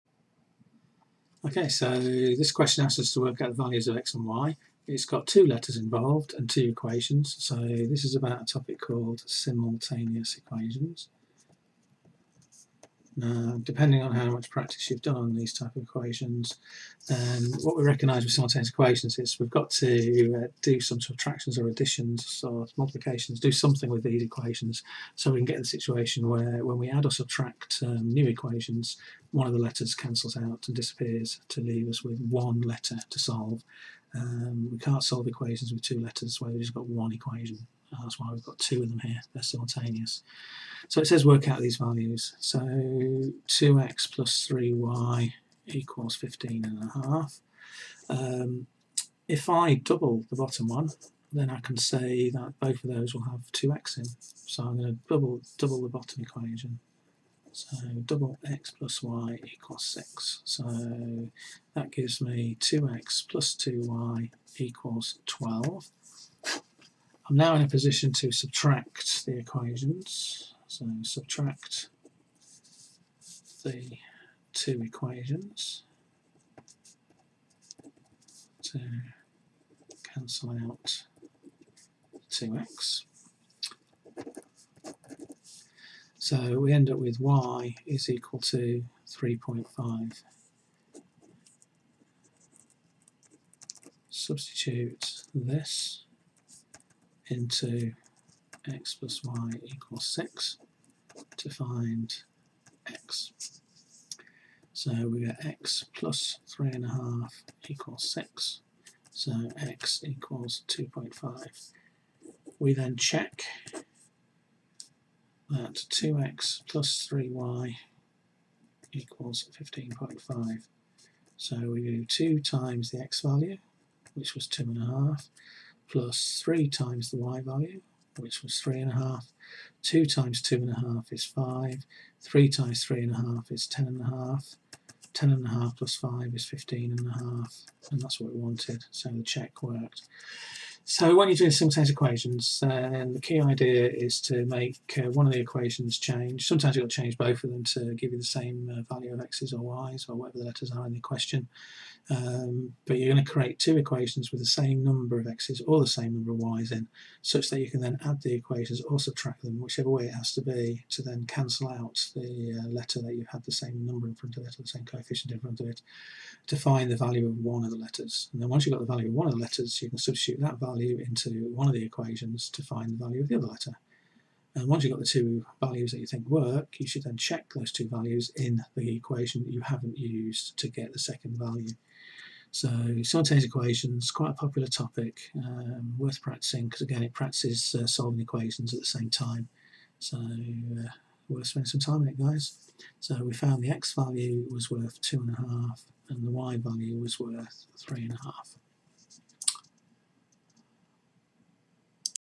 AQA GCSE 2017 Specimen Paper 1F Q28
This video covers the topic of solving a pair of simultaneous equations.